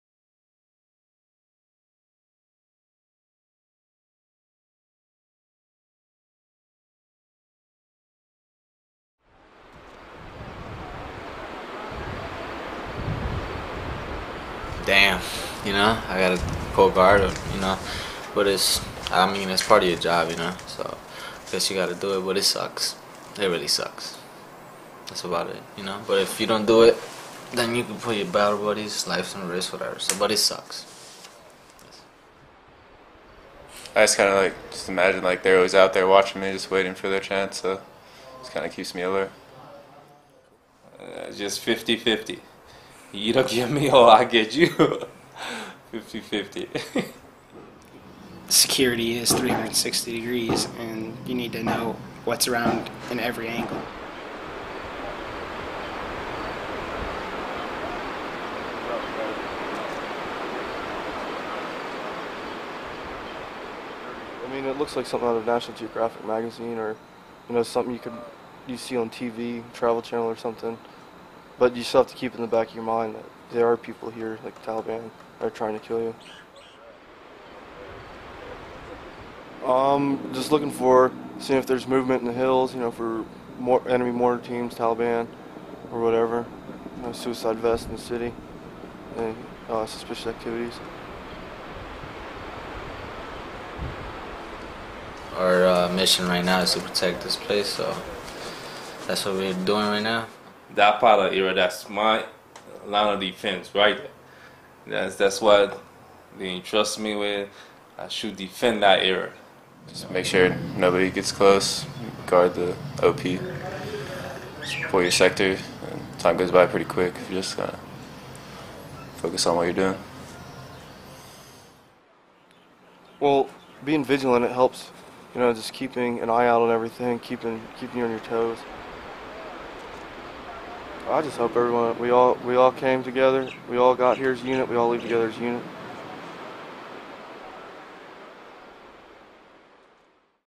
24 Hours at OP Coleman
A day in the life for Soldiers of Bravo Company, 2nd Battalion, 27th Infantry Regiment, Task Force No Fear, 3rd BCT, 25th Infantry Division, at Observation Post Coleman located where the Pech River Valley and Kunar River Valley merge in Afghanistan.